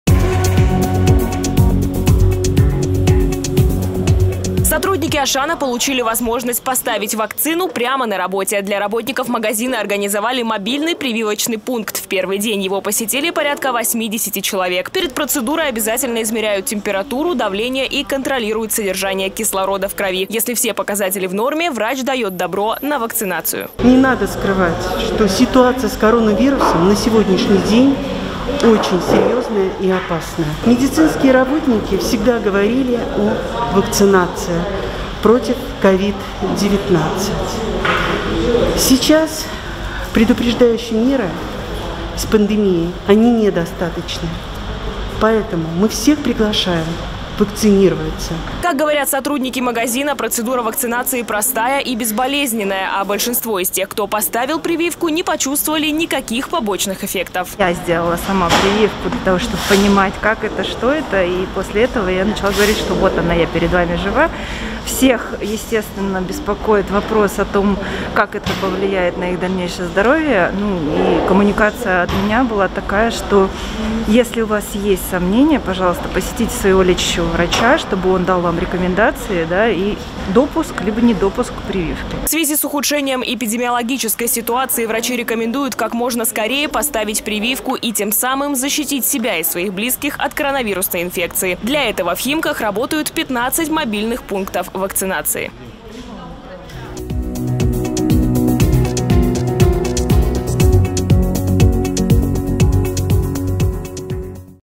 Сотрудники «Ашана» сделали прививки прямо на работе. 24.06.2021
Сотрудники «Ашана» получили возможность поставить вакцину прямо на работе. Для работников магазина организовали мобильный прививочный пункт. В первый день его посетили порядка 80 человек. Перед процедурой обязательно измеряют температуру, давление и контролируют содержание кислорода в крови. Если все показатели в норме – врач даёт добро на вакцинацию.